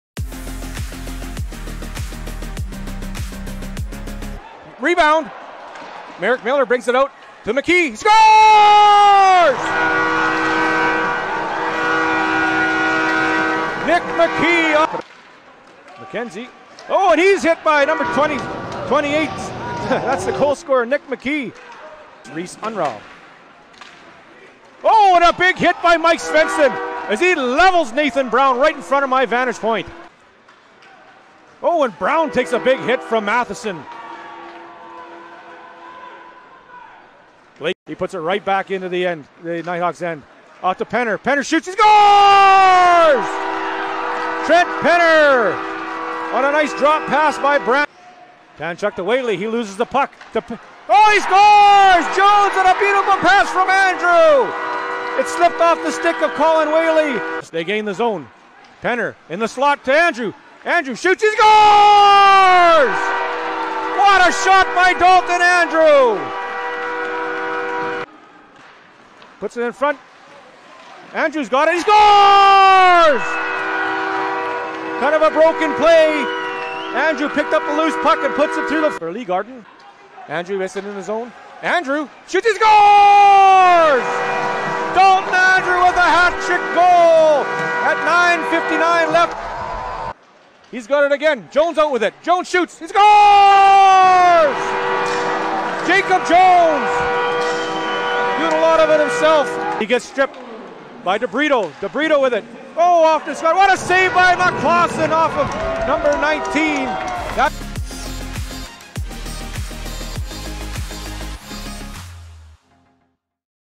Triple E Game Highlights - 01.05.2024 - Flyers vs Nighthawks
It was a 7 goal night on Friday at home against the Nighthawks! Here are some of the Triple E highlights from a great win, in front of an incredible crowd!